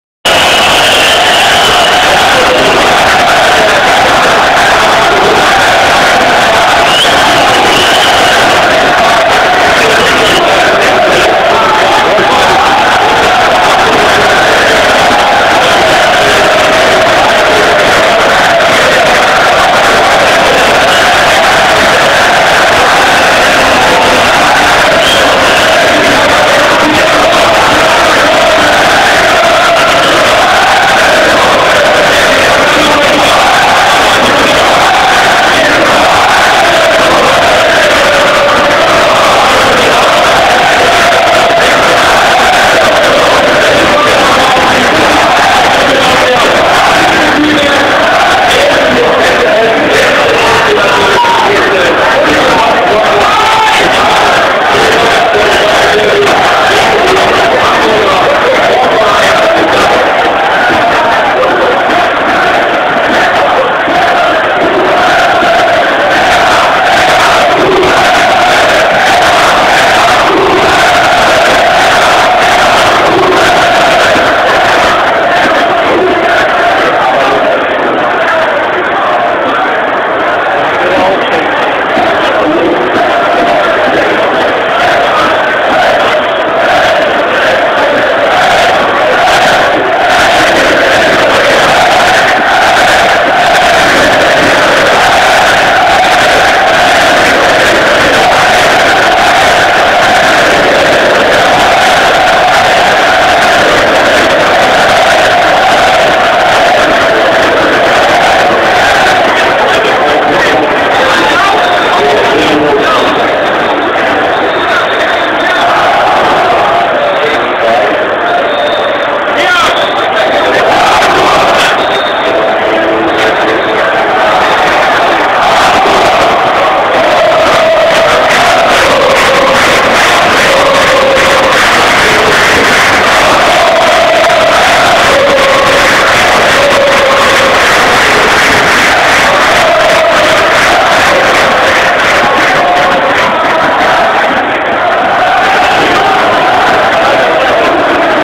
hsv partyblock vorm spiel
Der HSV block im hässligen weserstadion und das ne halbe std vorm speil